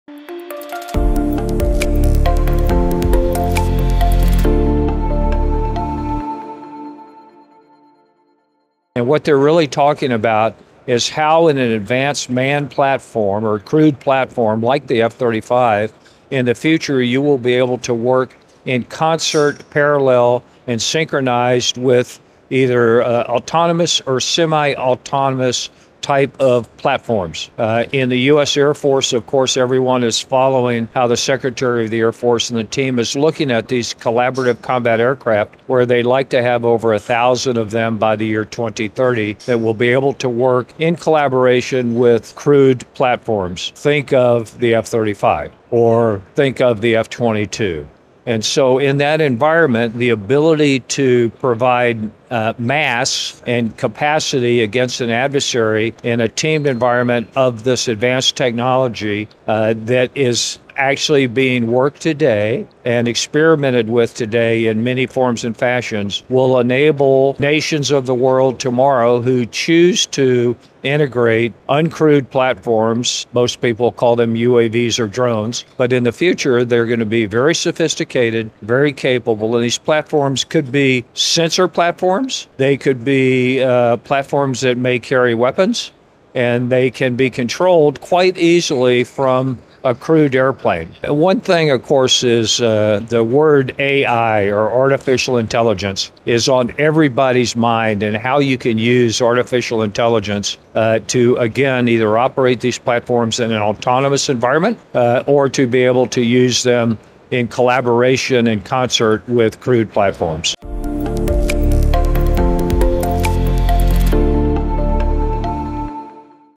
MSPO Kielce 2024: Collaborative Combat Aircraft (Lockheed Martin)
General Gary North, Lockheed Martin's Vice President and former combat pilot, confirmed to us that everyone within the U.S. Air Force is following the intentions of the U.S. Air Force Secretary and the entire team working on the CCA program to have over 1,000 of these drones capable of working with manned platforms such as the F-35 or F-22 by 2030.